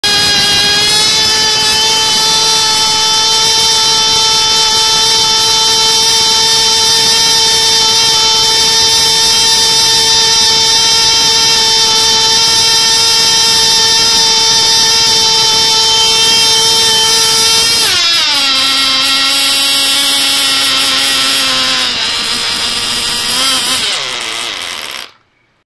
Cox Tee Dee RC .050 - All Carbon 4x4.5 / 27,051 rpm / 115 mph
Tee Dee RC .050 - All Carbon 4 x 4.5 speed prop.
Best static peak 27,051 / .164hp / 122w / 8.5oz thrust / 115 mph
Nelson plug / aluminum carb body / pressure tap on muffler.